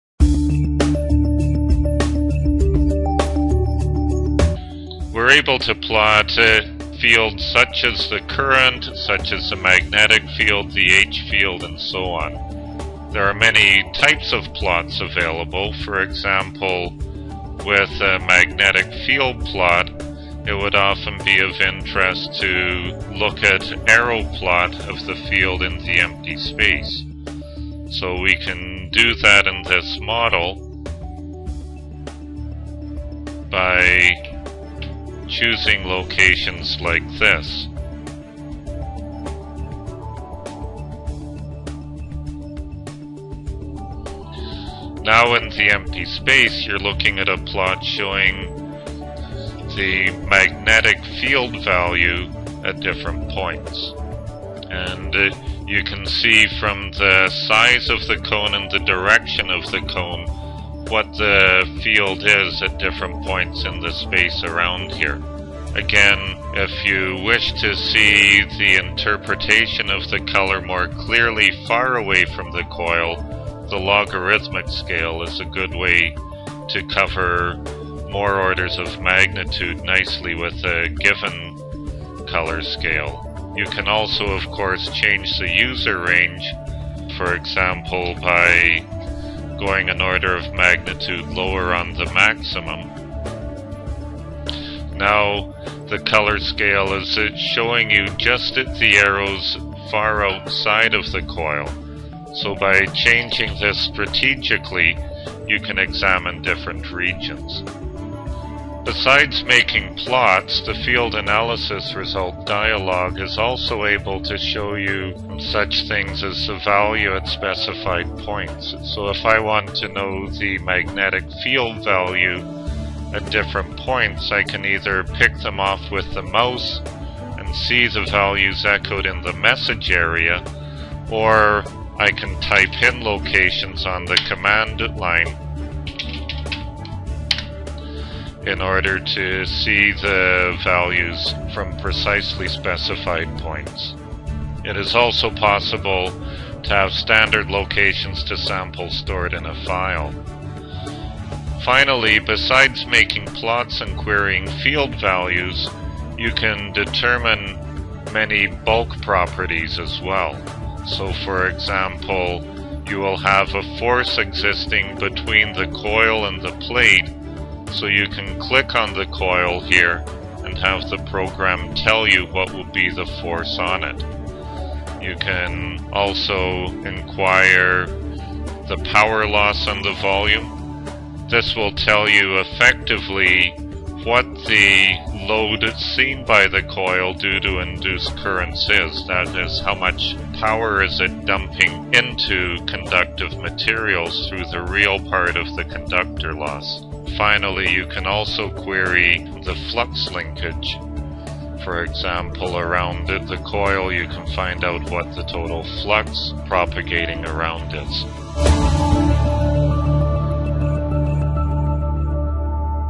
FARADAY Software - Overview 3
FARADAY v8.0 - Demonstration of Coil with Conductive Plate in FARADAY which is 3D Magnetic, Time-harmonic, Eddy Current simulation Software